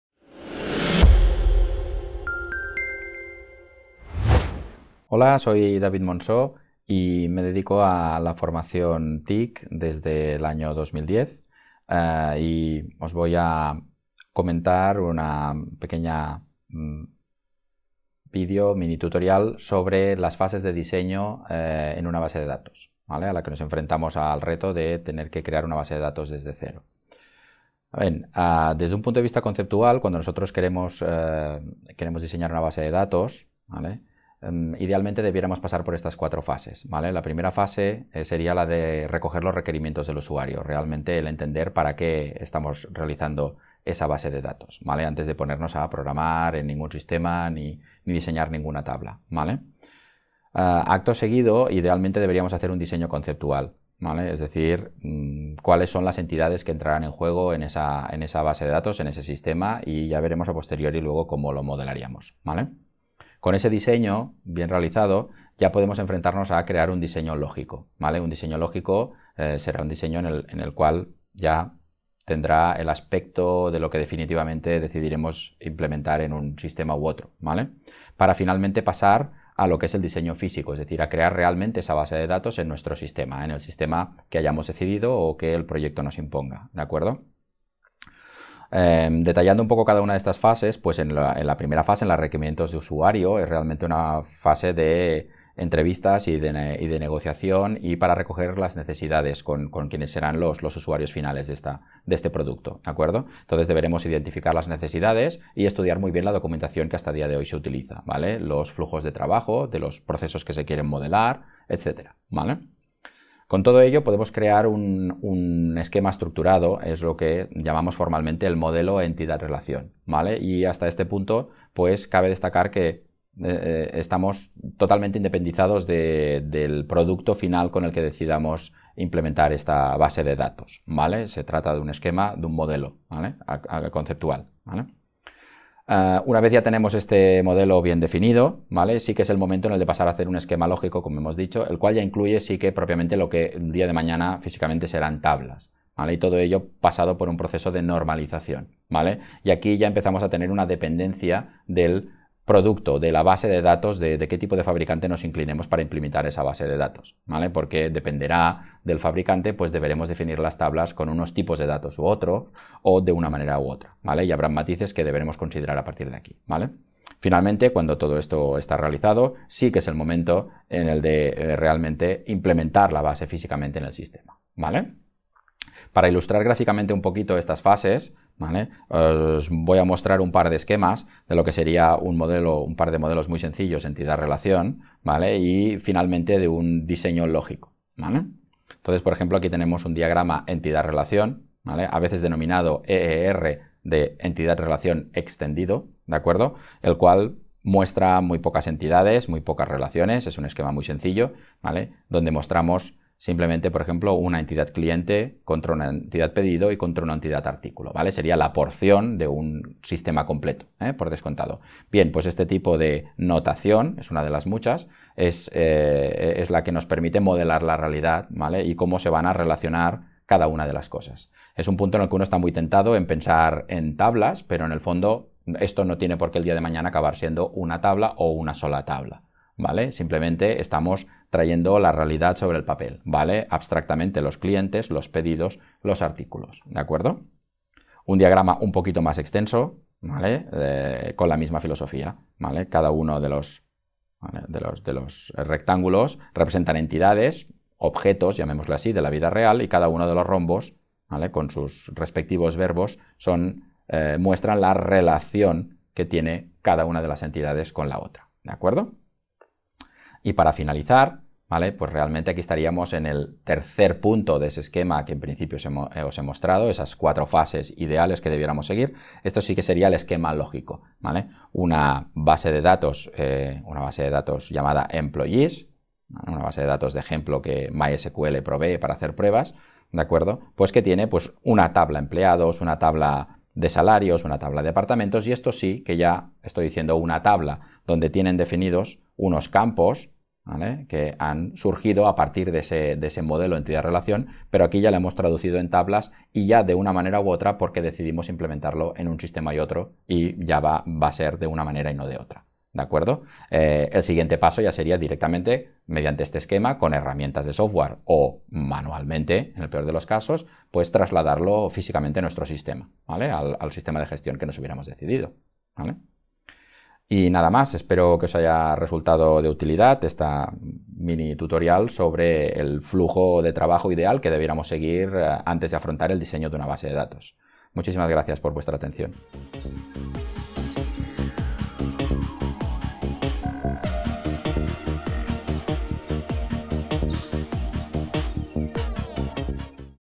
FASES DE DISEÑO DE UNA BASE DE DATOS
Cuando tenemos que crear o diseñar una base de datos, tenemos que hacer un diseño conceptual de lo que necesitamos, ya que no es tarea fácil crear una base de datos desde cero.

Para ello hay que pasar por 4 fases, conócelas y empieza a crear bases de datos profesionales.